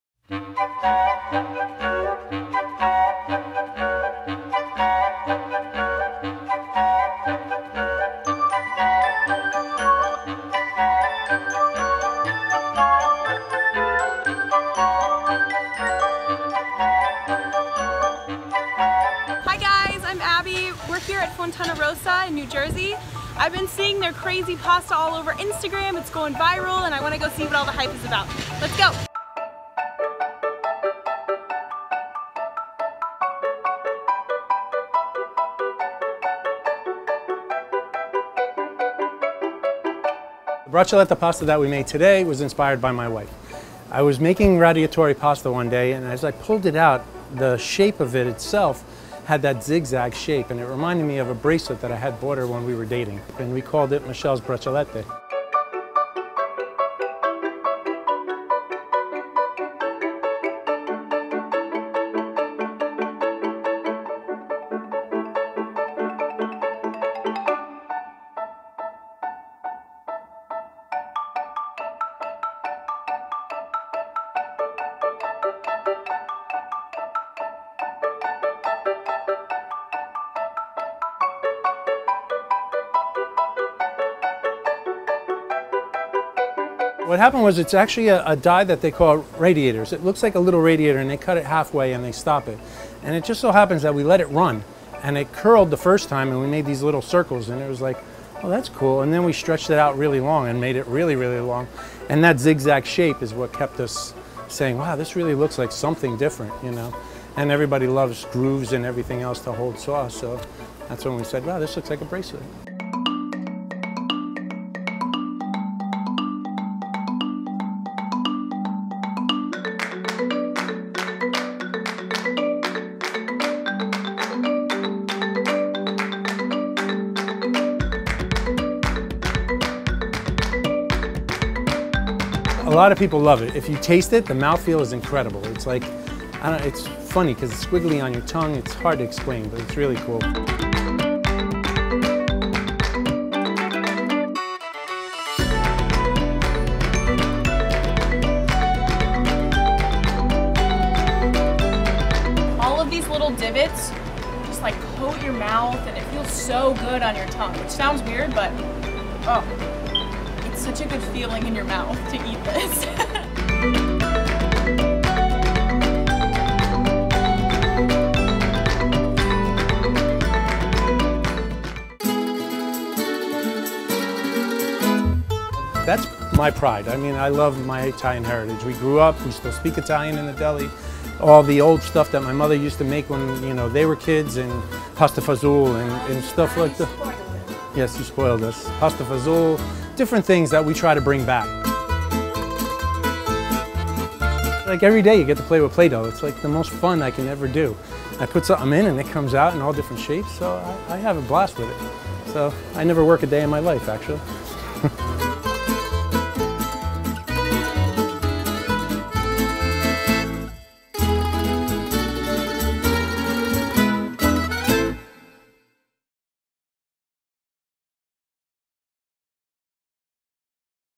How "Bracelet" Pasta Is Made
Fontanarosa's Gourmet Speciality Foods in NJ makes braccialetto or "bracelet pasta." The unique-looking pasta is made like its more common counterparts: flour and water are mixed together and formed in a machine. It can even be stretched to 100 feet! Braccialetto is best served with a simple tomato and garlic sauce.

FOOD INSIDER believes life should be a delicious adventure.